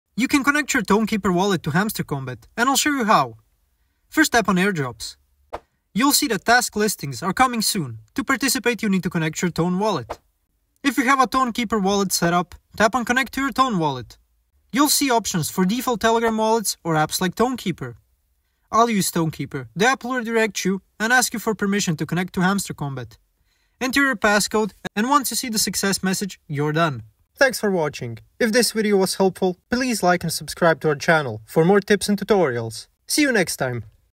HOW TO QUICKLY CONNECT TON KEEPER WALLET TO HAMSTER KOMBAT (2024)
Want to connect your TON Keeper Wallet to Hamster Kombat? 🐹⚔️ In this full 2024 guide, I’ll show you how to quickly link your TON Keeper wallet to Hamster Kombat step-by-step, so you can start playing and managing your in-game assets without any hassle.